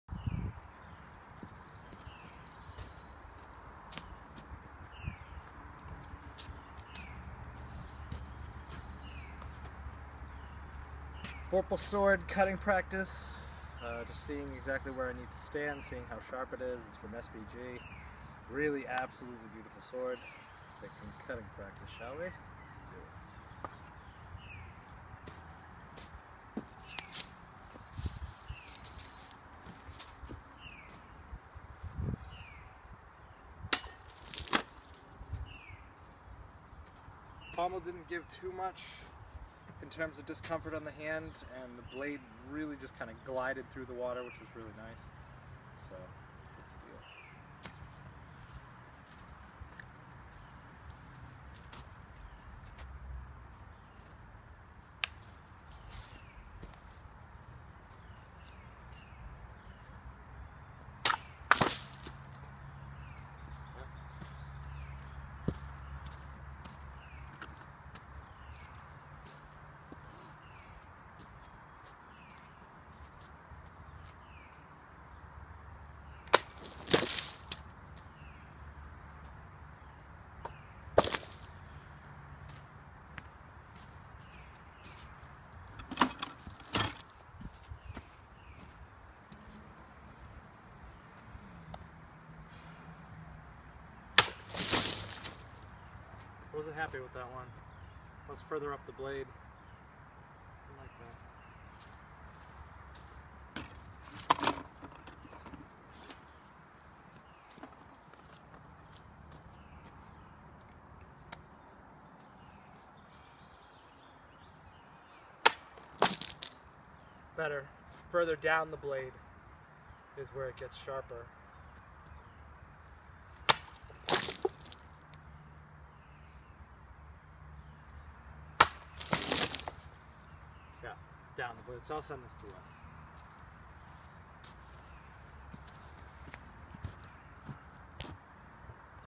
SBG Vorpal Sword Cutting
This fantasy blade was made by BCI International and the Sword Buyer's Guide (SBG). It's sharp, deadly, and fast. Take a look!